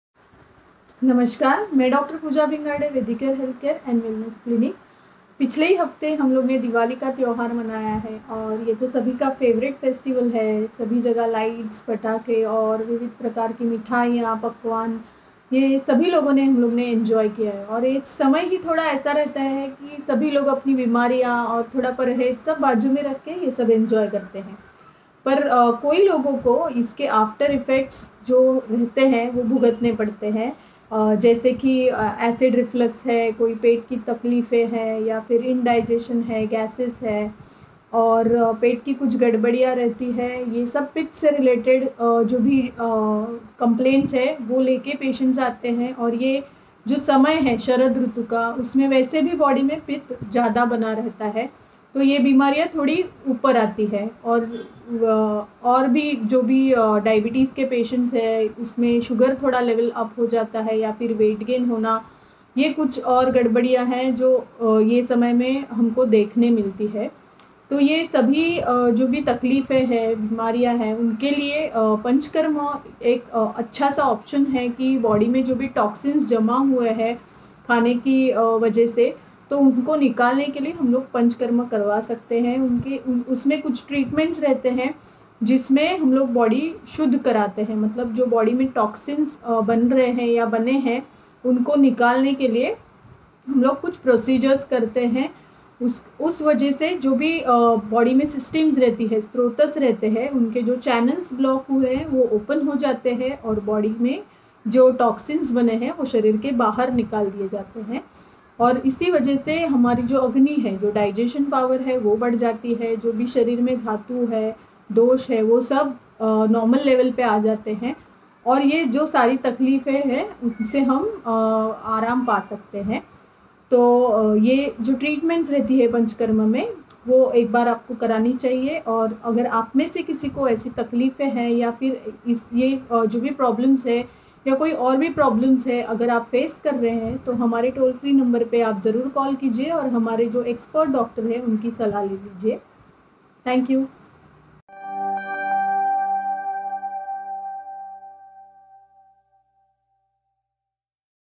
SampurnSharirShuddi - How to do a Post-Diwali Detox at Home - by Dr Pooja Bhingarde
People often complain about bloated stomach, acidity or constipation after the festivities, because weak digestive system and poor body metabolism cannot manage the excess intake of sweets and deep-fried food.
In such situations, people can adopt a detox diet to flush out the toxins from the body while giving good rest to their liver and kidney.  Are you looking for some detoxification after indulging in too many sweets this Diwali? Here are some expert tips that can help you detoxify after the Diwali binge.

For more query or appointment:-